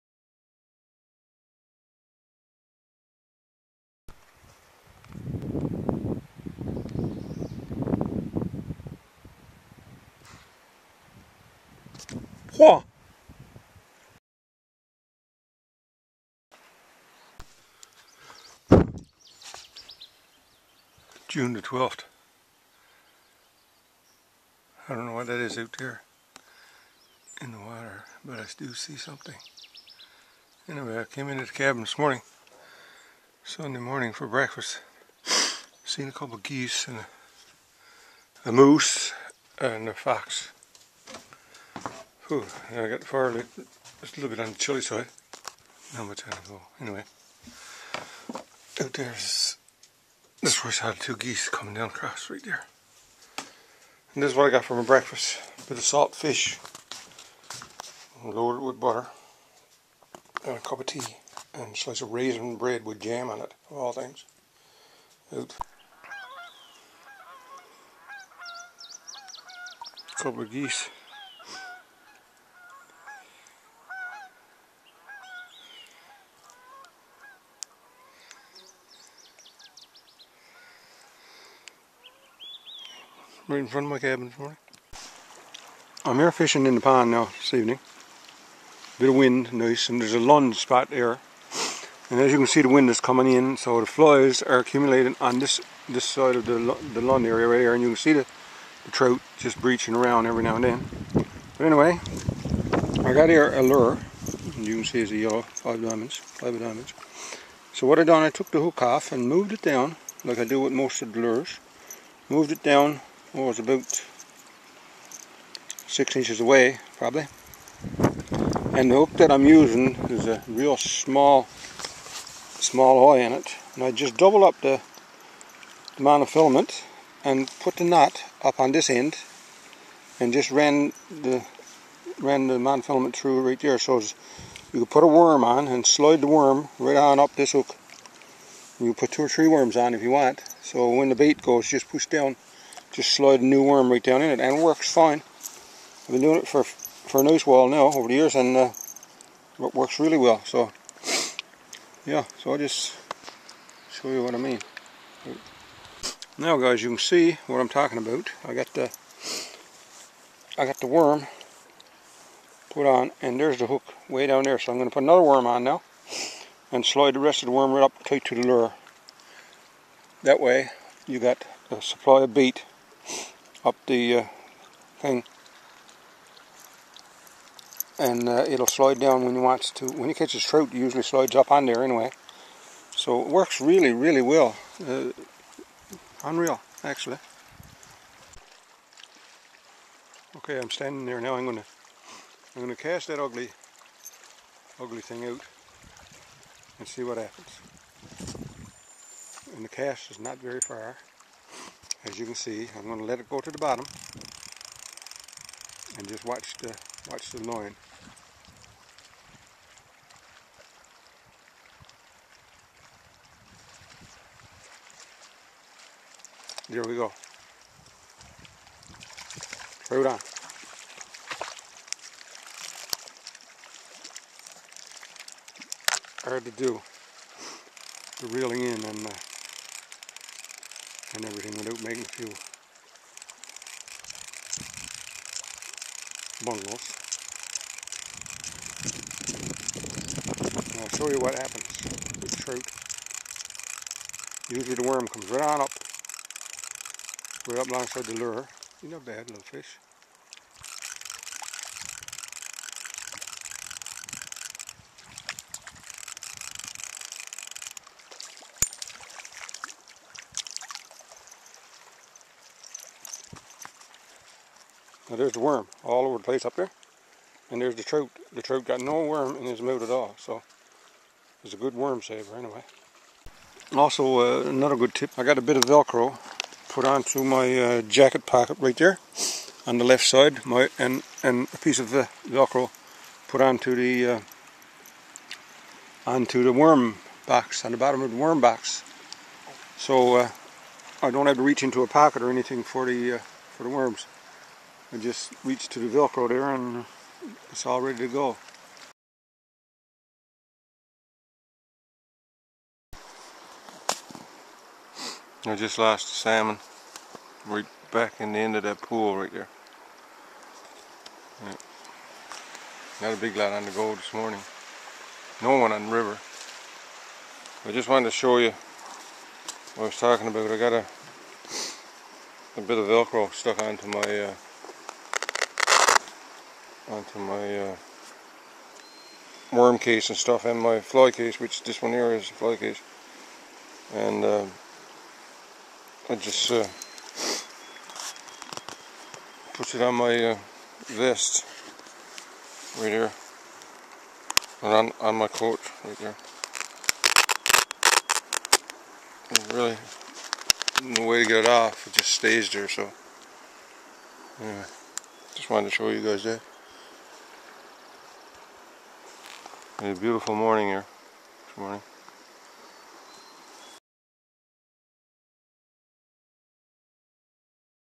Couple fishing tips and tricks
Doing a little bit of fishing and couple tricks and tips I use to make life a little easier , hope you like them!